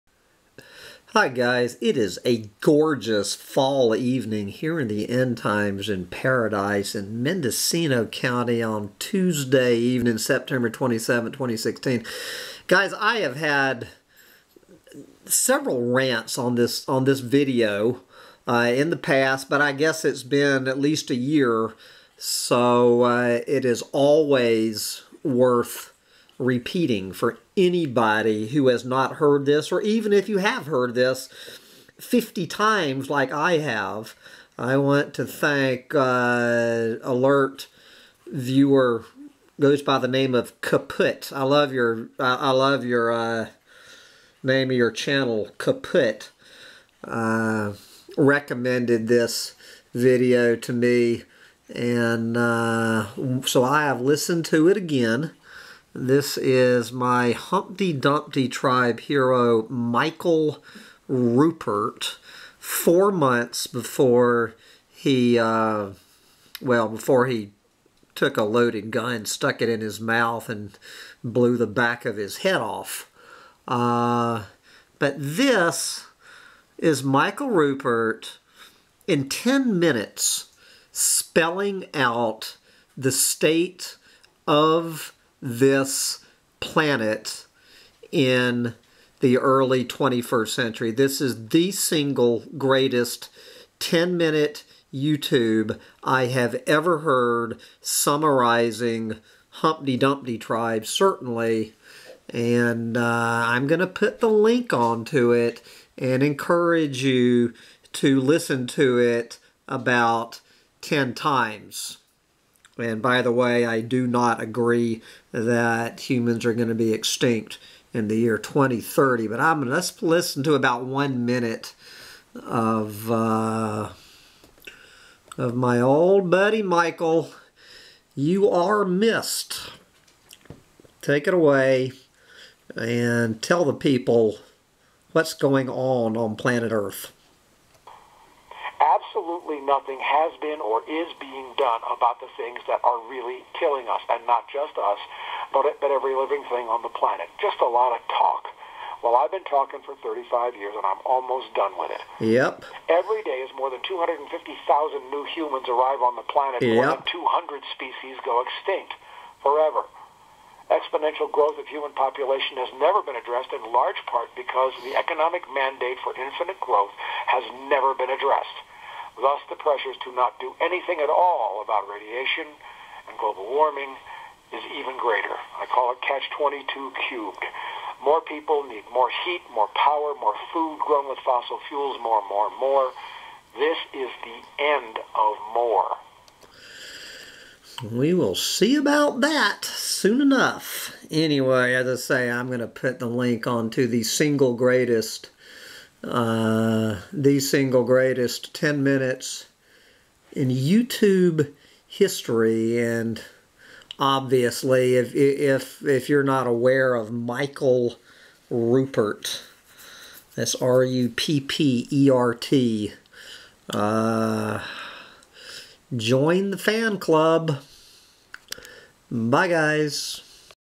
The Single Most Spot-On Youtube Video in History (RIP Michael Ruppert)
For the upteenth time, your old Doomsday prophet shares what I believe to be the single most spot-on analysis of the state of this planet I have ever encountered on Youtube, compliments of the late great Michael C. Ruppert, about four months before he blew his brains out.